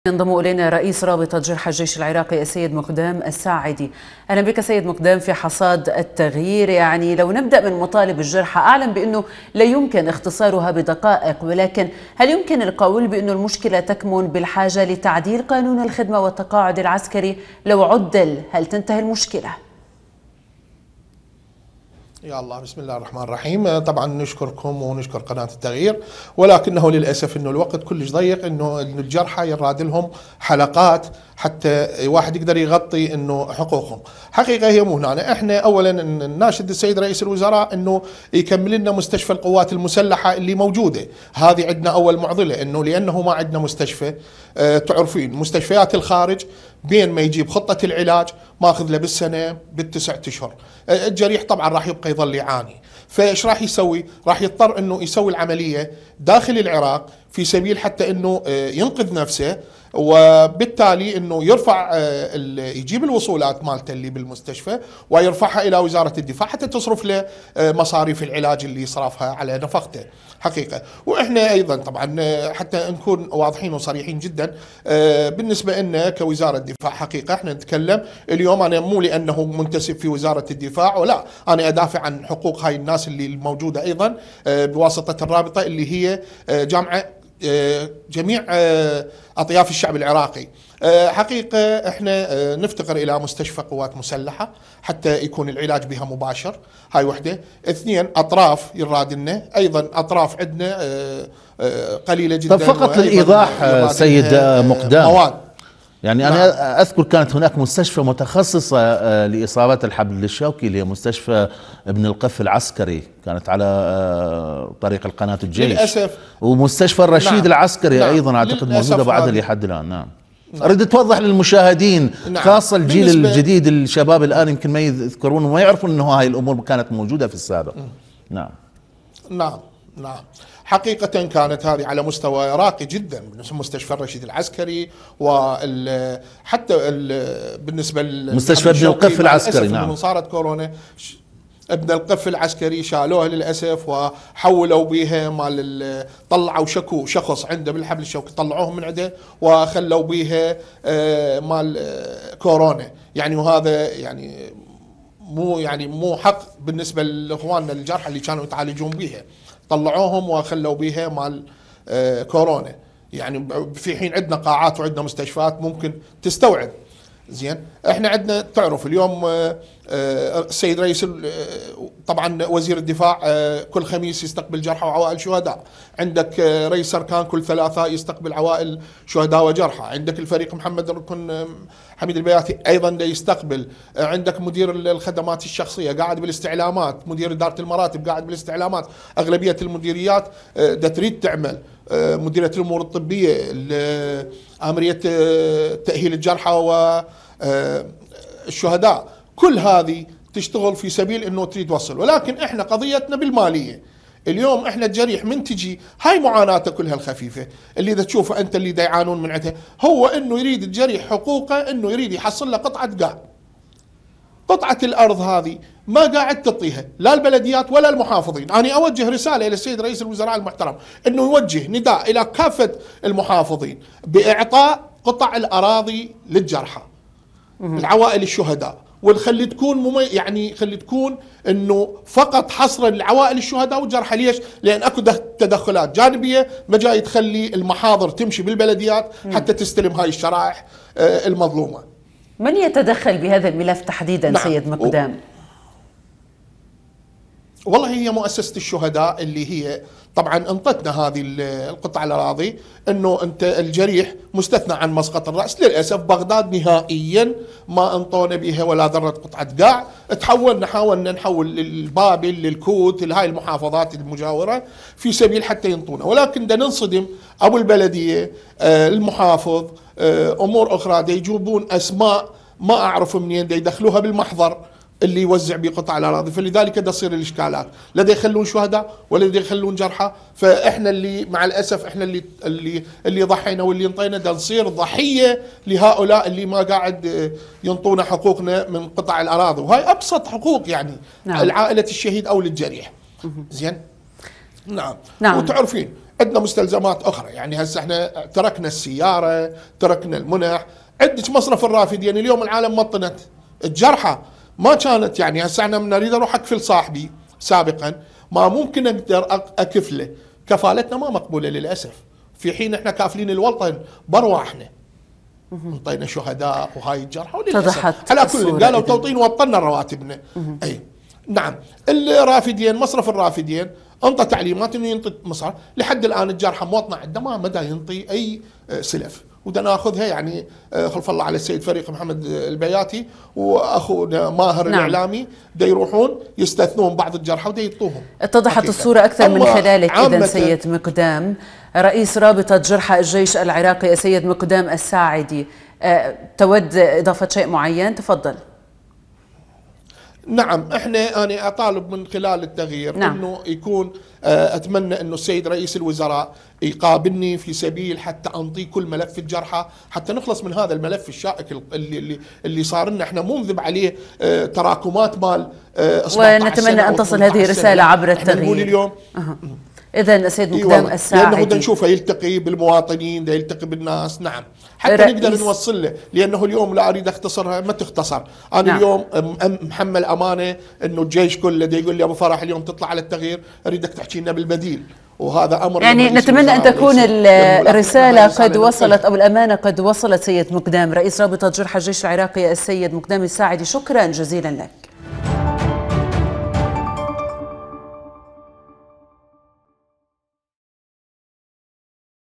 الساعدي يروي معاناة الجريح في الجيش العراقي
موقع قناة التغيير الفضائية الالكتروني